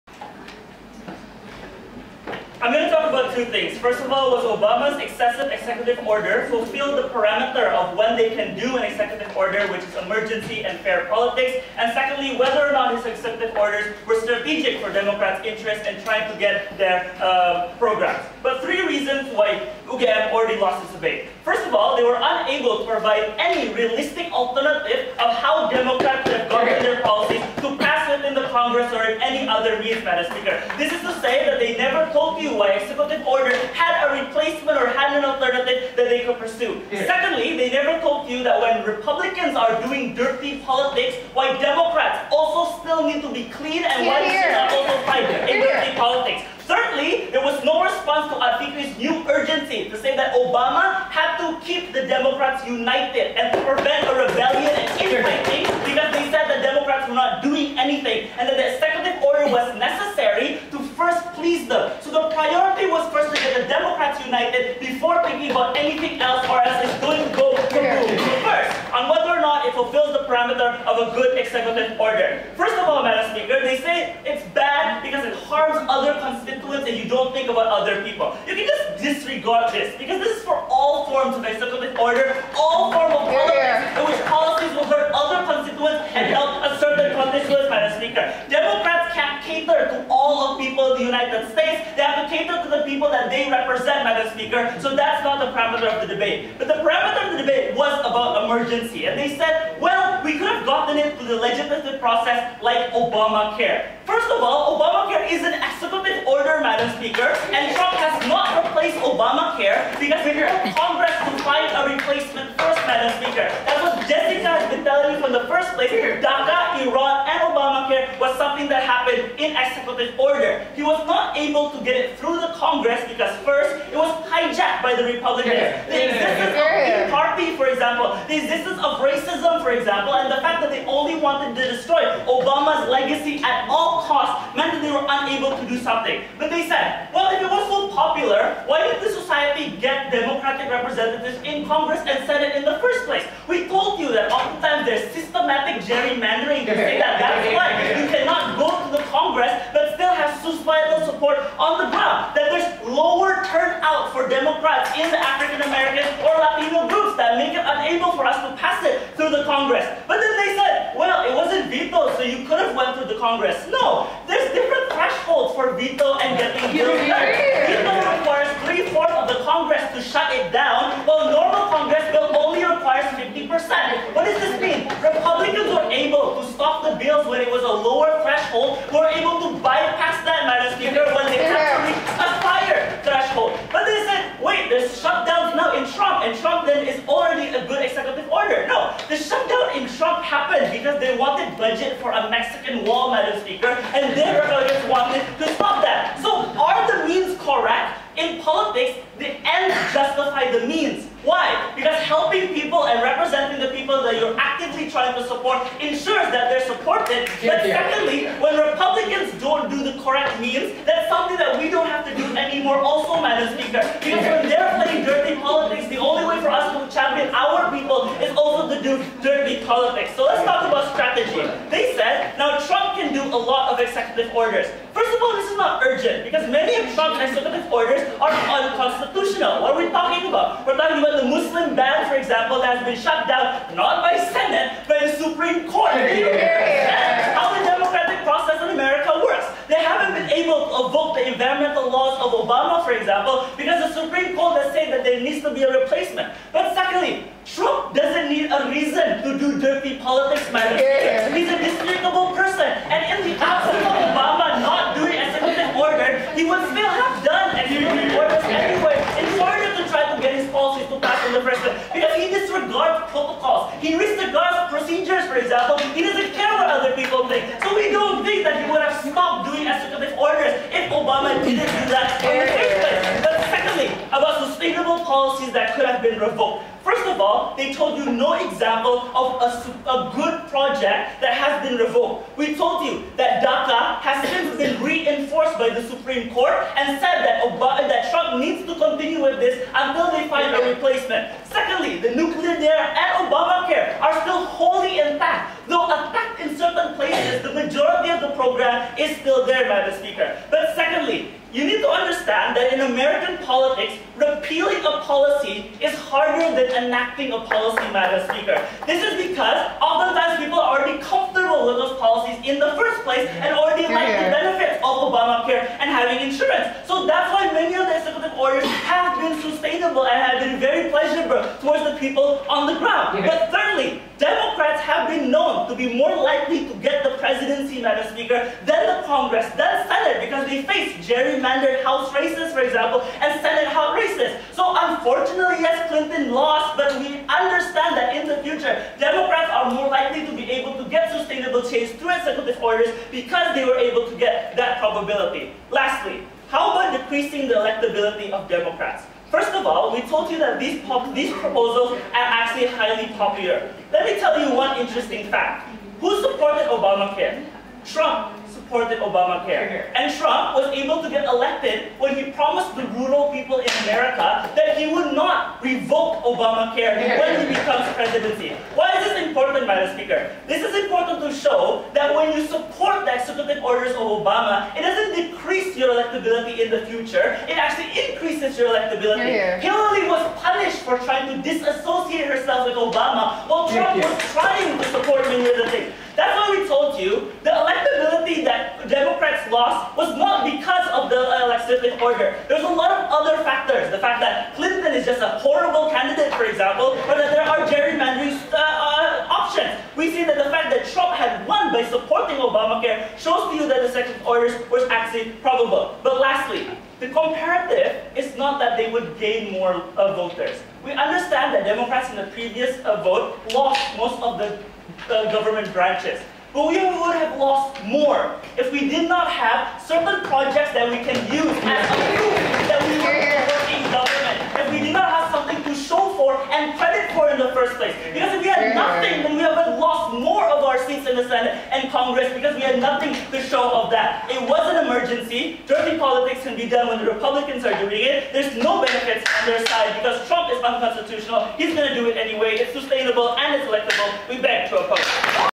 (6/8) Alif - Grandfinal of ALSA UI 2018
As Democrats, This House Regrets Obama's Excessive Use of Executive Order

As Proposition, from UGM A:
1. Noel
2. Lia
3. Haikal

As Opposition, from UI A:
1. Jessica
2. Adfikri
3. Alif